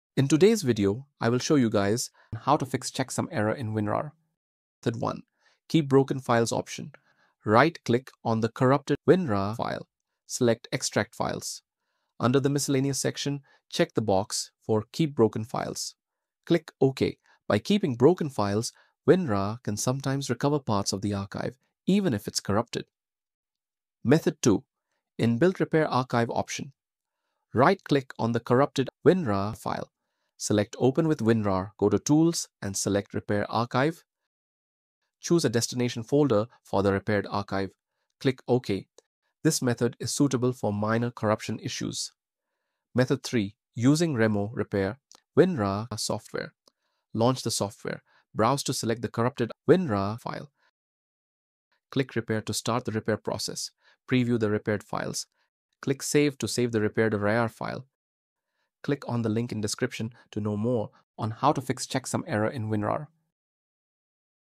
FIXED: Checksum error in WinRAR
This video will help you fix the annoying Checksum error in WinRAR